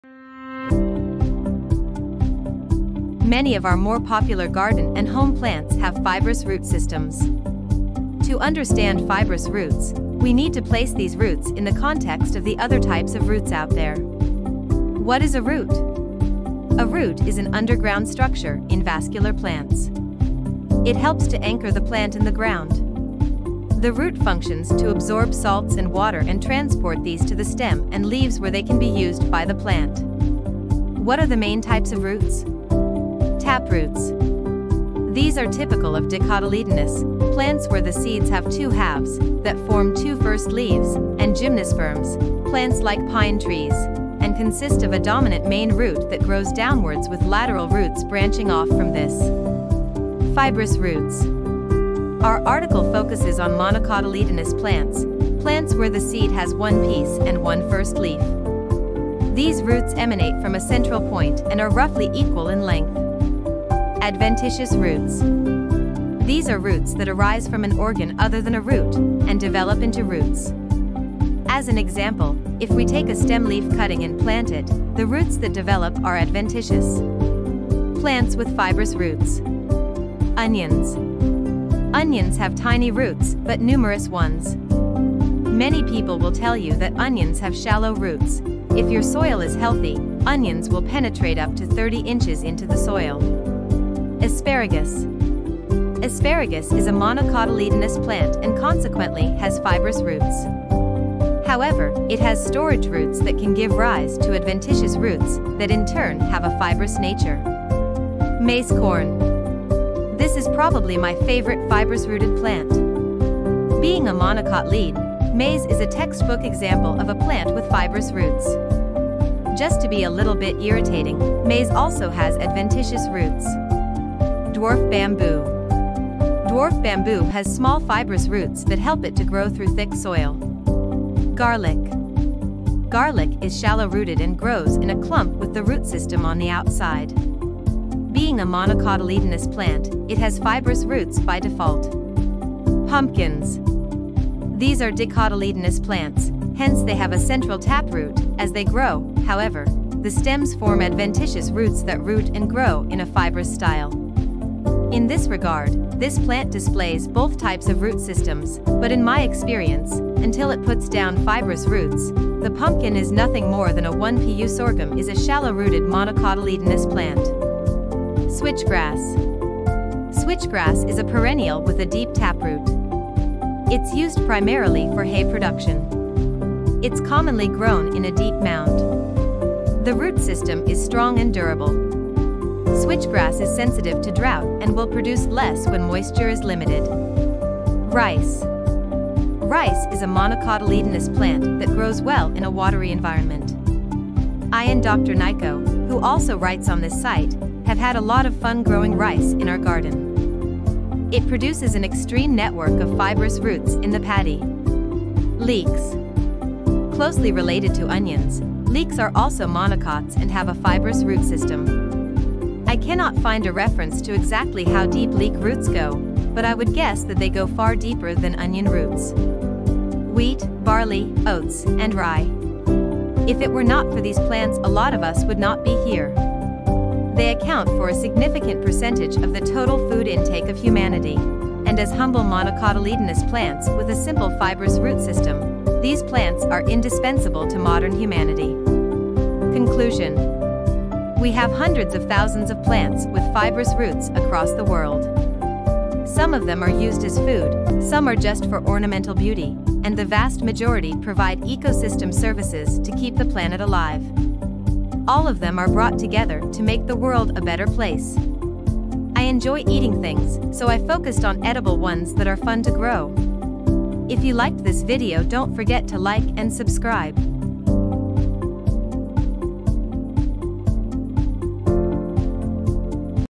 15 Plants With Fibrous Roots
Many of our more popular garden and home plants have fibrous root systems. To understand fibrous roots, we need to place these roots in the context of the other types of roots out there.